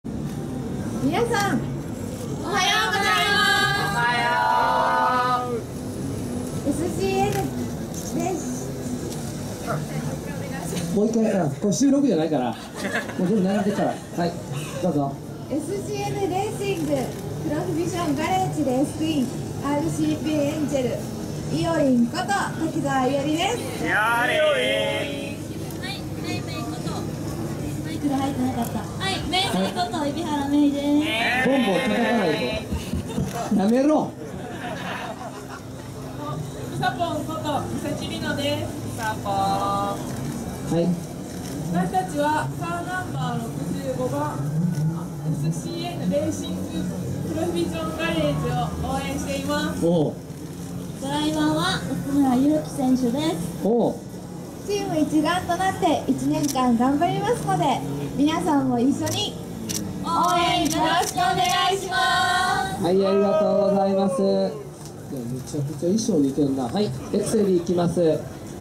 2016 D1GP Rd.1 & Ex. お台場⑦ 美人レースクイーン 三崎りのさん 蛯原メイさん 滝沢いおりさん RCPS ANGEL
2016年3月26日～27日にお台場・東京都江東区青海特設会場で開催されたイベント、2016 D1グランプリ イン お台場 第1戦&エキシビジョンマッチ( 2016 D1 GRAND PRIX SERIES Rd.1 & EXHIBITION MATCH )の会場を盛り上げてくれたRCPS ANGELの美人レースクイーンさん：三崎りのさん 蛯原メイさん 滝沢いおりさんの動画です。9:40～のイベント広場でのレースクイーン紹介にて。